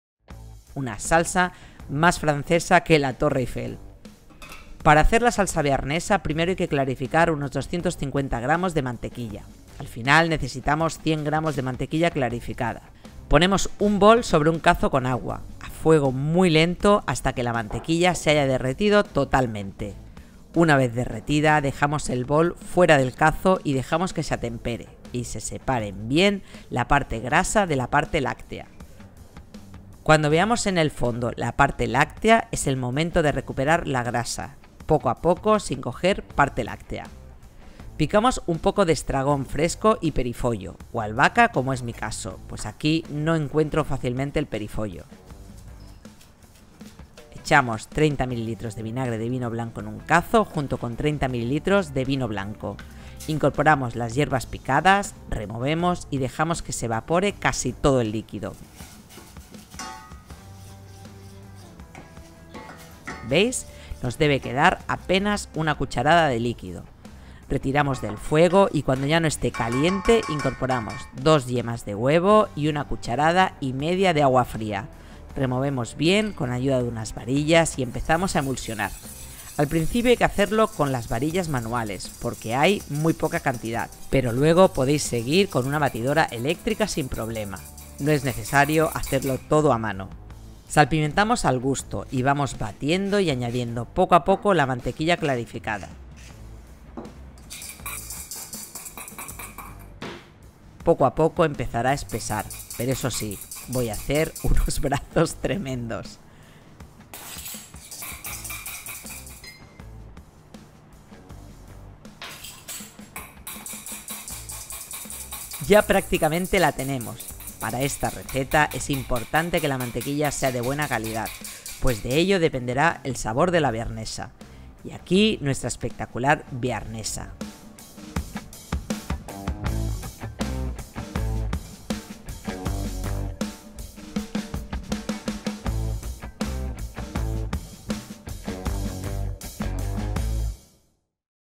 SALSA BEARNESA - Receta
Cómo hacer una salsa bearnesa para tus platos de carne y pescado. Receta de la Larousse Gastronomique.

Ingredientes:

100 g de mantequilla clarificada
30 ml de vino blanco
30 ml de vinagre de vino blanco
2 yemas M
30 ml de agua fría
1 chalota
Estragón fresco
Albahaca o perifollo
Pimienta negra
Sal

2021